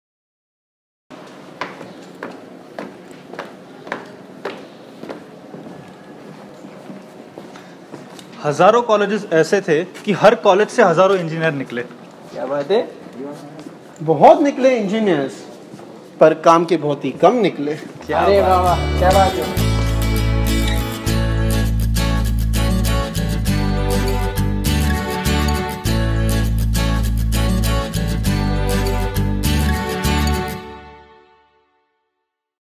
YD - Year Down | Hazaaro khwahishein aisi | ADVOLVE
There Is A Need For Good Engineers But There Is Definitely No Need Of Any More Engineering Students.
Every Year, A Sea Of Engineers Pass Out From Engineering Colleges In India, But The Quality Of Engineering They Are Capable Of, Is Hilarious.
Hence Life Of An Engineer In India, Is A Struggle Between Success And Hard Work..!
Life Of These Students Revolves Around Creating Situational Shayari..!

To Know More About These Guys, You Can Buy This Book From Amazon.